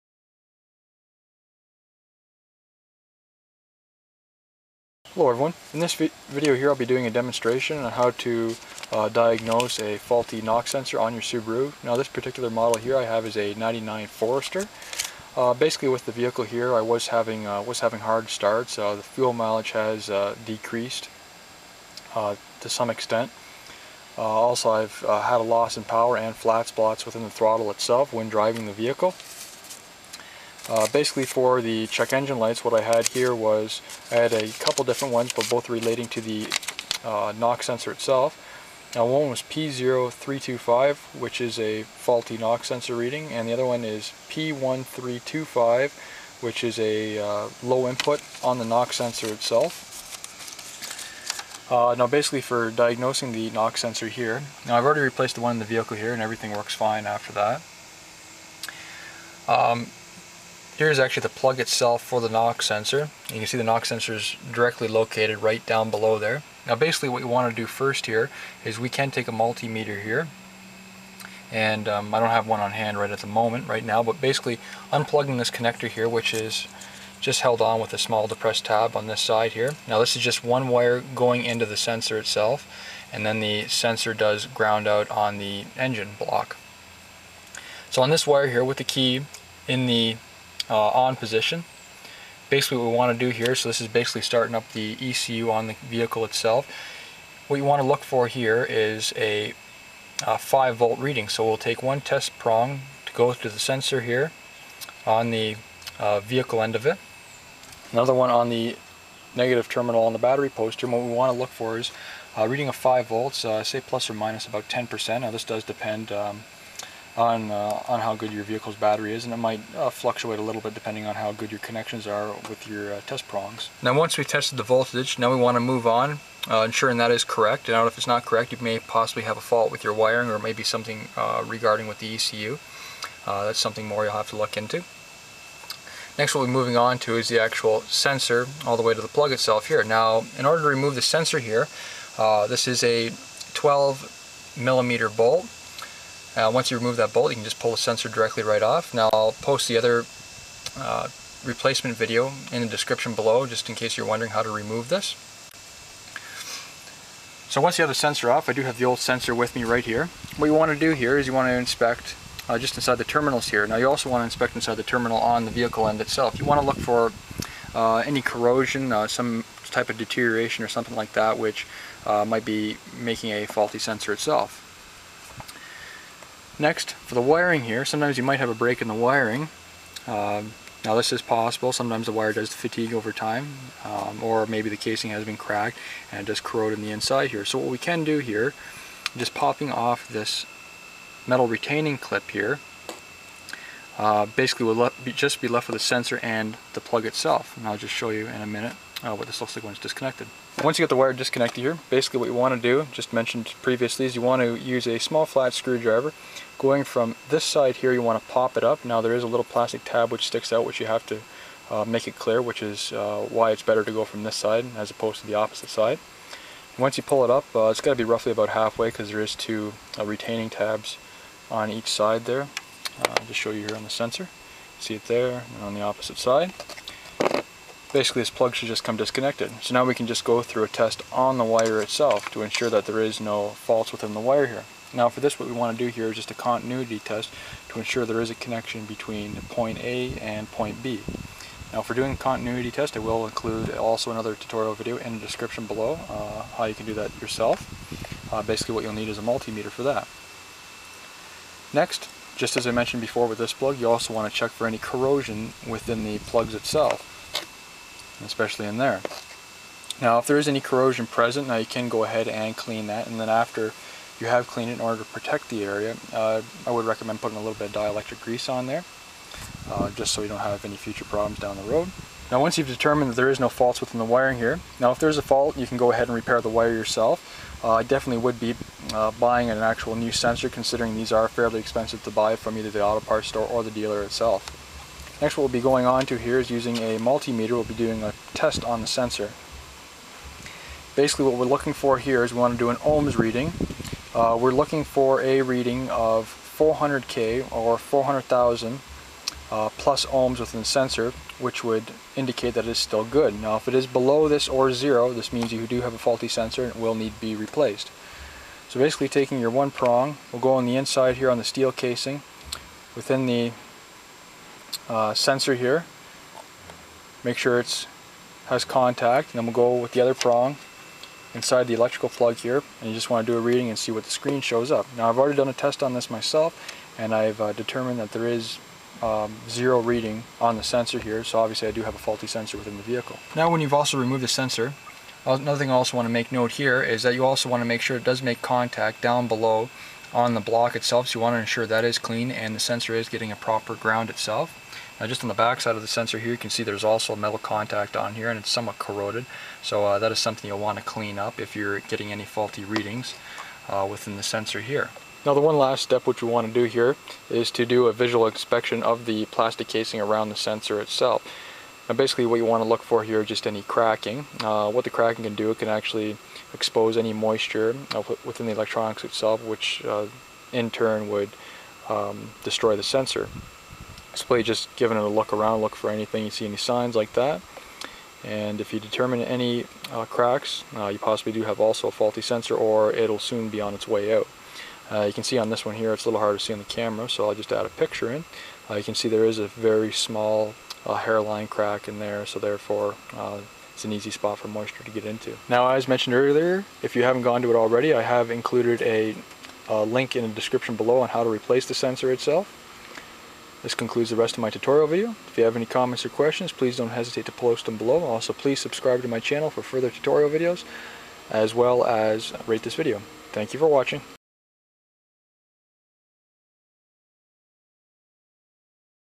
DIY: How to Test a Subaru Knock Sensor
Video tutorial on how to diagnose a faulty knock sensor on a Subaru.  A faulty knock sensor will typically cause poor fuel mileage, decrease in power, flat spots in the throttle, throttle hesitation, and hard starting. It is important to test the knock sensor first as they can be fairly costly to replace. This particular video was done on a 1999 Subaru Forester with a 2.5L 4 cylinder.

Tools/Supplies Needed:
-multimeter
-12mm 3/8" driver socket
-3/8" drive 3" and 6" extension
-3/8" ratchet
-torque wrench

Procedure:
-first check for any breaks or corroded wiring/plug connections
-you can do a continuity test to determine if there is a fault wire within the circuit
-check for a clean ground connection between the sensor on the engine block
-check for any cracks in the casing of the sensor which would make it susceptible to moisture
-disconnect the wire
-using an multimeter, set it to the voltage setting, testing the harness on the ecu/vehicle side
-ensure there is an approximate 5V reading when the key is in the on position
-removing the sensor, using a 12mm socket, along with a 3" and 6" extension, you can access the bolt easily
-using a multimeter in the ohm setting to testing the sensor
-it should have a reading of 400,000ohm (400k ohm) or more which will determine that the sensor is still good
-ensure that the sensor is install in the proper position which is 60 degrees in accordance with the rear of the engine
-if the sensor does fail at any of the tests above, then it will need to be replaced
-reinstall the bold and tighten it to 24nm or 17.4ft lbs
-reinstall the wiring harness ensuring it isn't rubbing on anything